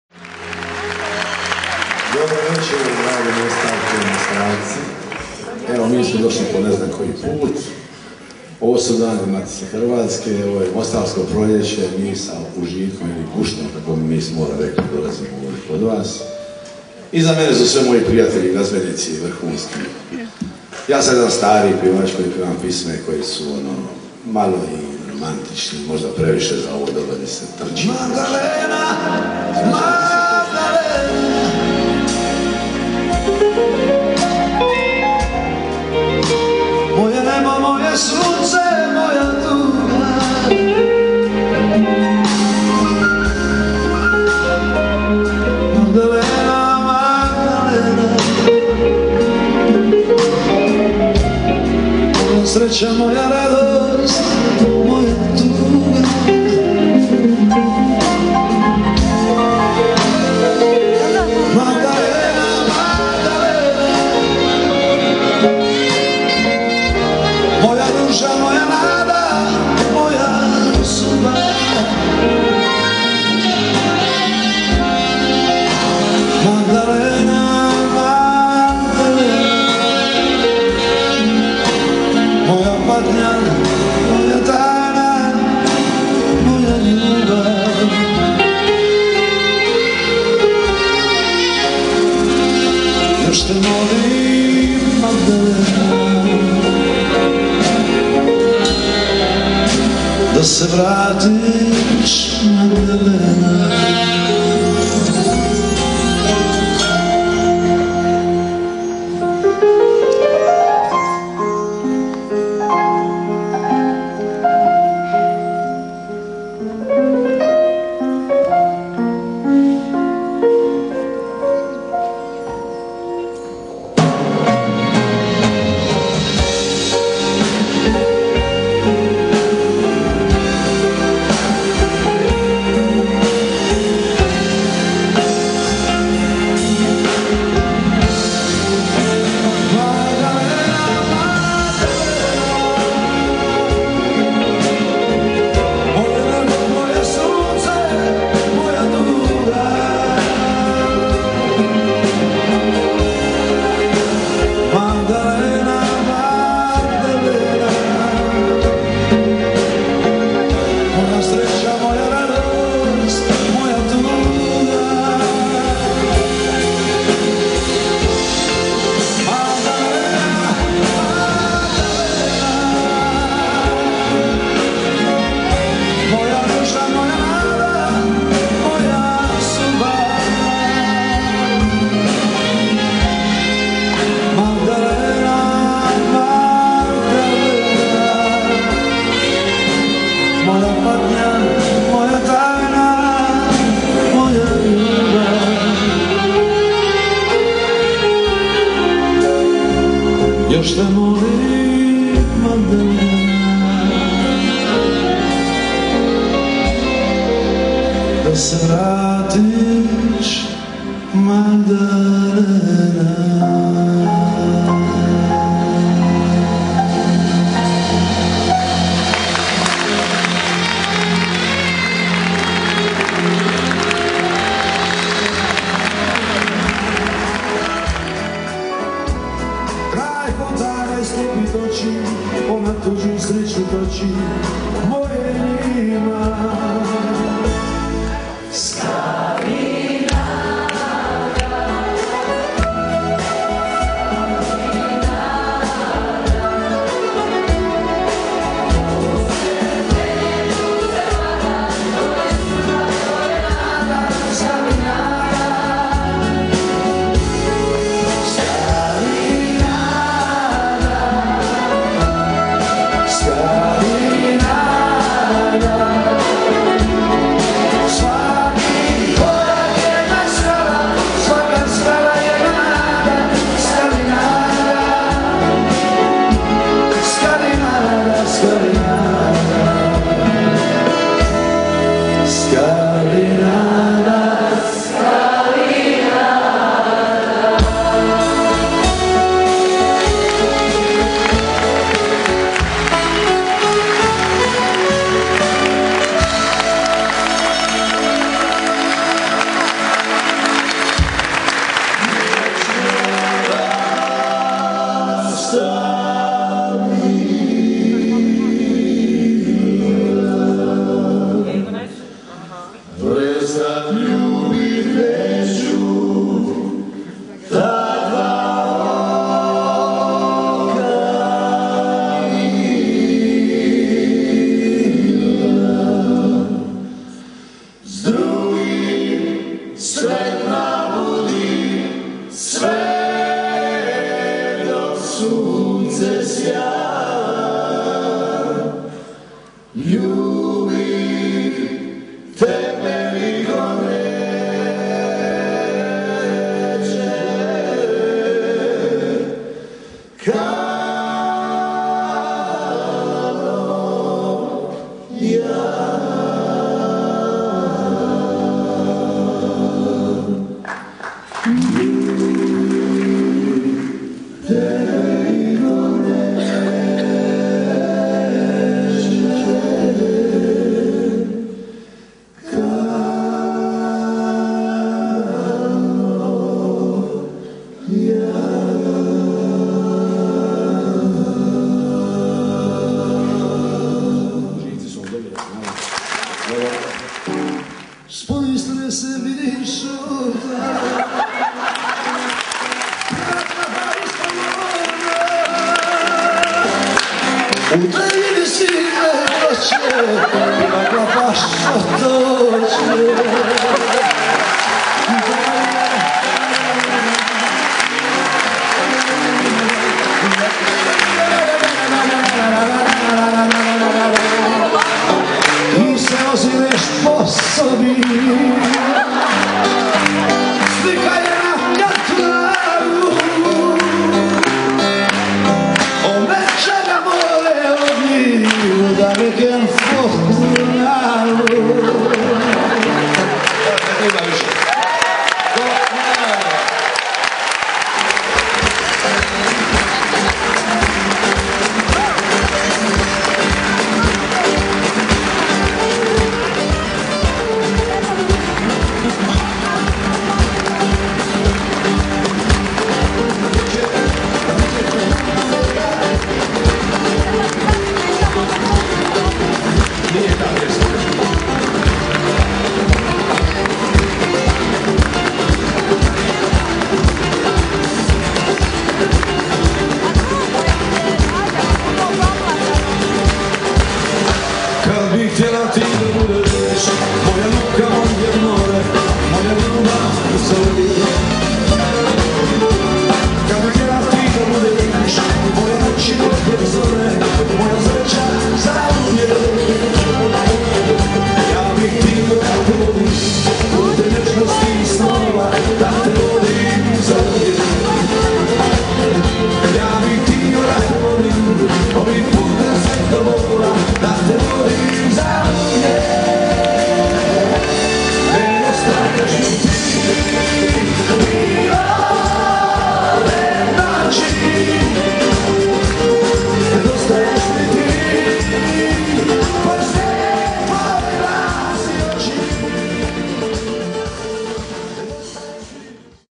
OLIVER DRAGOJEVIĆ - MOSTAR - HERCEGOVINA
Nastup Olivera Dragojevića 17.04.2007. u prepunoj dvorani Hrvatskog doma Hercega Stjepana Kosače u Mostaru u sklopu Dana Matice. Karte su u sekundi bile rasprodane, na jedvite jade smo se dokopali par njih, a svima koji nisu uspjeli doć na koncert poklanjamo nekoliko minuta ovog spektakla putem filmića.